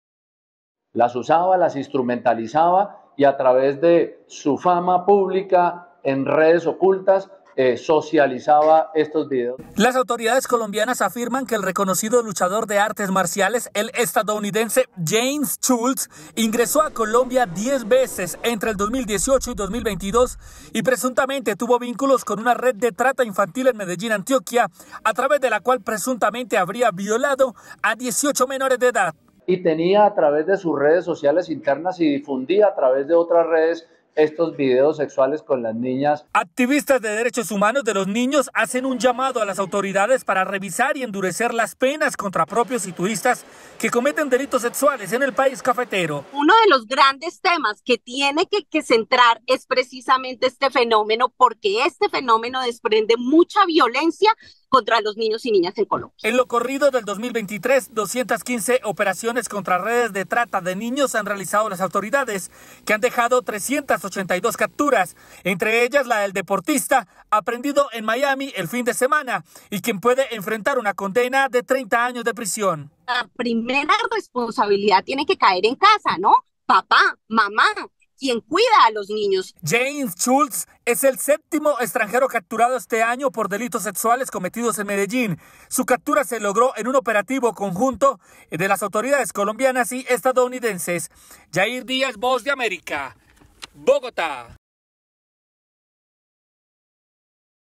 Luchador estadounidense acusado de pederastia en Colombia
En Colombia las autoridades acusan a un estadounidense, reconocido luchador de artes marciales, de presuntamente abusar de 18 menores. Jair Díaz, periodista de la Voz de América, con el reporte.